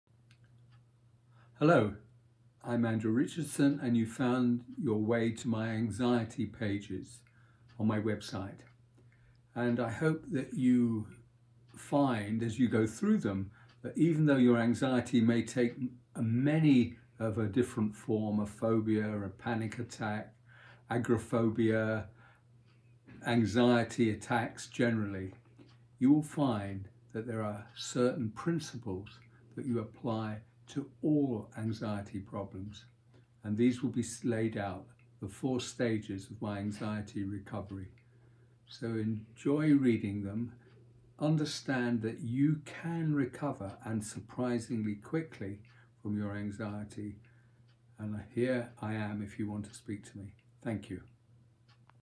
Overcoming Anxieties with Andrew M Richardson
A short introduction on my tools and tartare third on how to overcome your anxieties